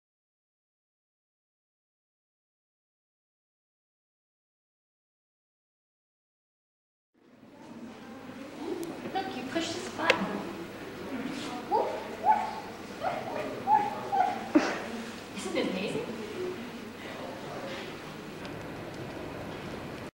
napoleon's dog or how to piss off a Parisian musem guard
Kitty Mervine demonstates an improvement for the Napoleon's dog display a the Military Museum in Paris.  What isn't shown is the very irate security guard.  You don't mess with Napoleon or his dog!